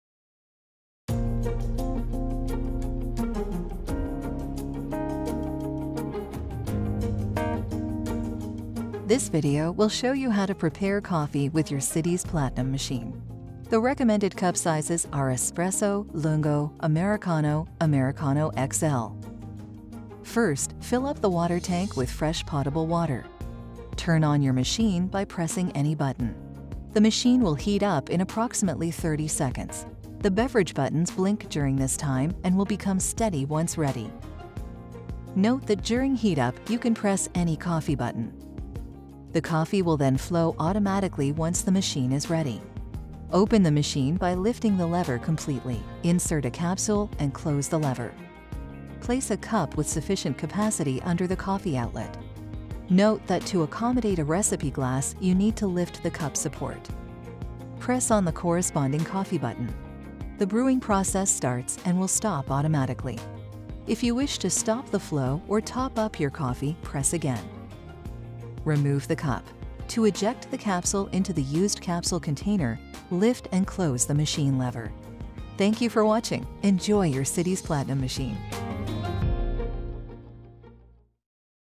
Citiz Platinum - Coffee Preparation
Your step by step Nespresso assistance video to guide you through the coffee preparations with your Citiz Platinum machine.
for more information. Please note that the machine used in this video is only available in specific countries.